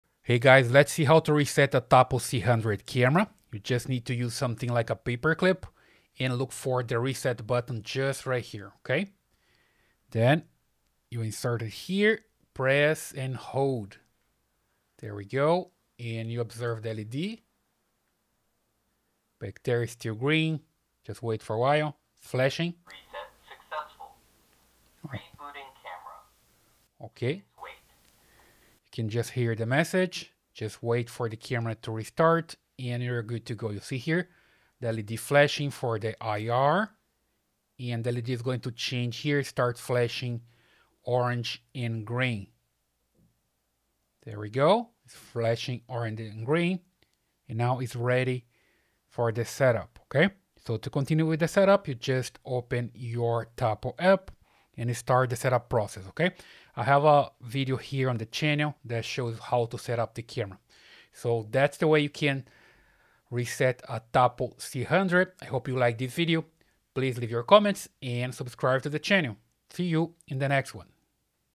How to Reset Tapo C100 Camera (TP-Link) QUICK and EASY
How to Reset Tapo C100 | Quick & Easy Camera Reset Guide

Wondering how to reset your Tapo C100 camera? In this step-by-step tutorial, I’ll show you exactly how to perform a full reset on your Tapo C100 — perfect for when you change Wi-Fi, troubleshoot connection issues, or prepare the camera for a new setup.

📌 What you’ll learn:

How to locate the reset button on the Tapo C100
How to use a paperclip (or similar tool) to reset
How long to press and hold the reset button
LED light behavior to confirm the reset
How to know when the camera is ready to reconnect

✅ Works for Tapo C100 indoor Wi-Fi camera
✅ No special tools needed — simple manual reset
✅ Compatible with Tapo App (iOS & Android)

Whether you're changing Wi-Fi networks or troubleshooting your Tapo C100, this quick reset guide will help you get your camera working again.